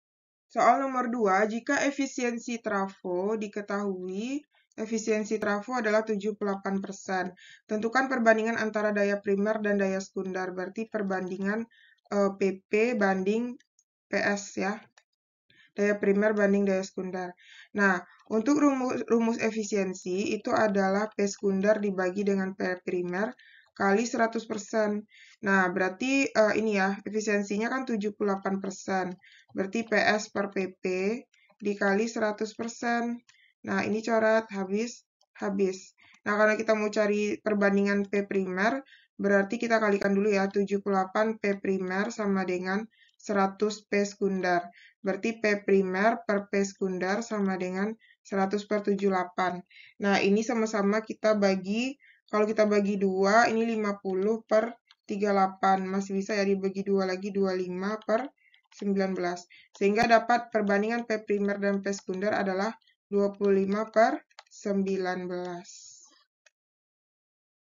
LS INDUKSI ELEKTROMAGNETIK no 2 LKS TUNTAS
jika efisiensi trafo 78 % tentukan perbandingan antara daya primer dan daya sekunder adalah